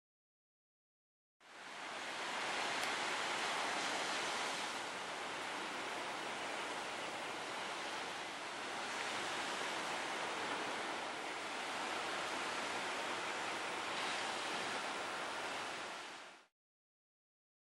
Lightning in Thunderheads
The thunderheads are too far away to hear the thunder resulting from these lightning flashes.